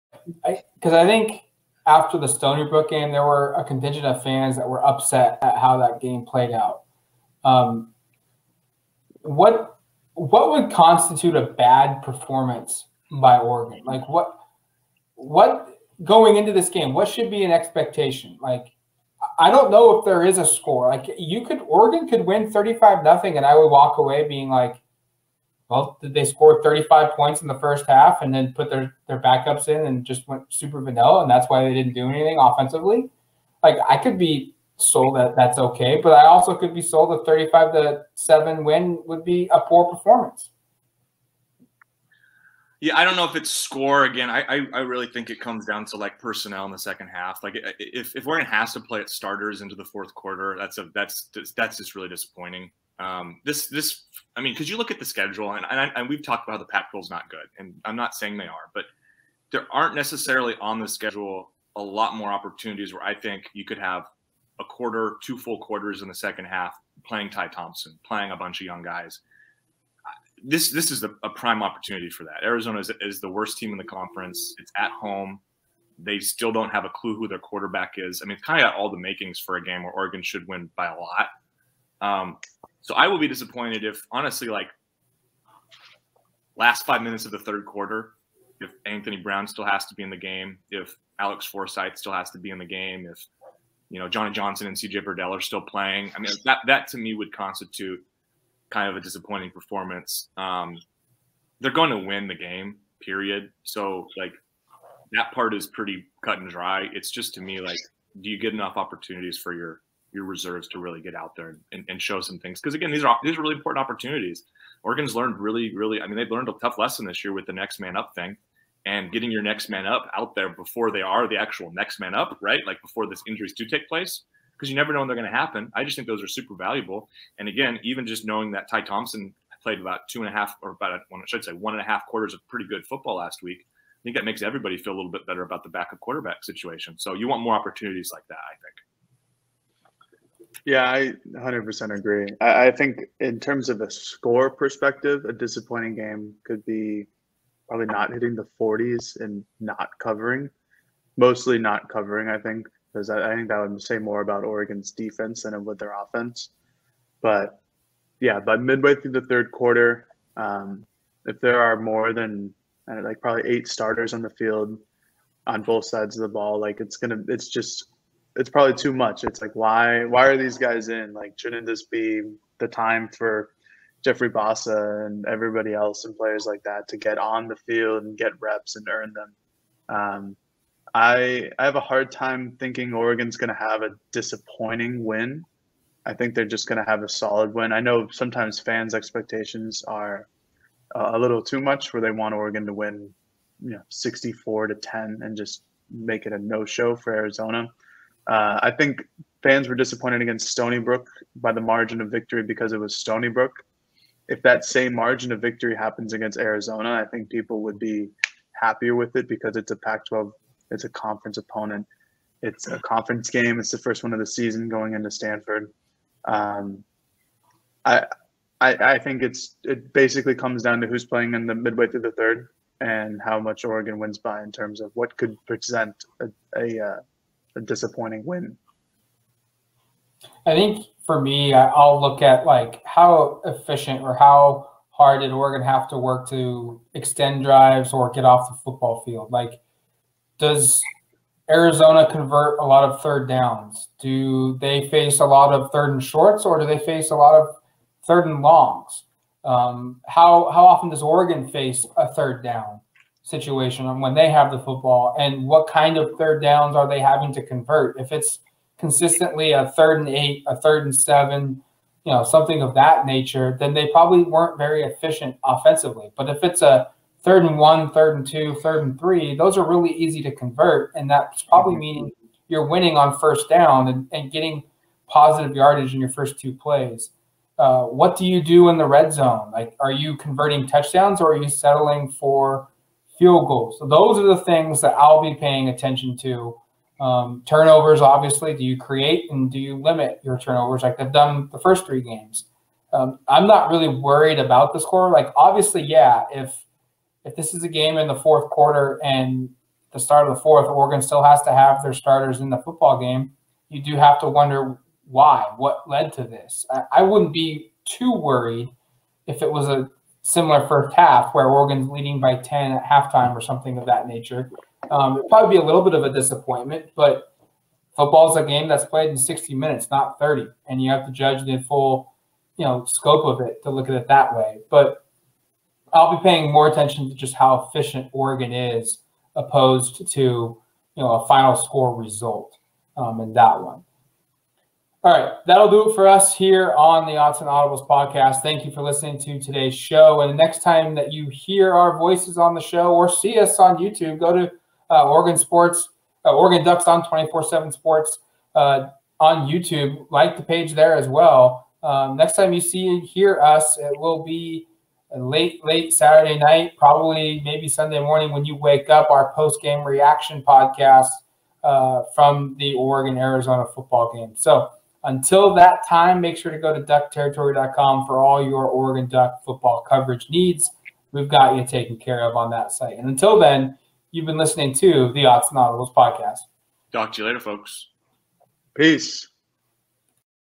How do you define a good win for Oregon as a 28-point favorite vs Arizona?
The Oregon Ducks are going to be massive favorites over visiting Arizona this weekend at Autzen Stadium. The Ducks are 28.5 point favorites according to most sportsbooks. So, is it simply covering the point spread to define a good win by Oregon or are there other ways to look at this game and what a potential win could be looked back on as a good win?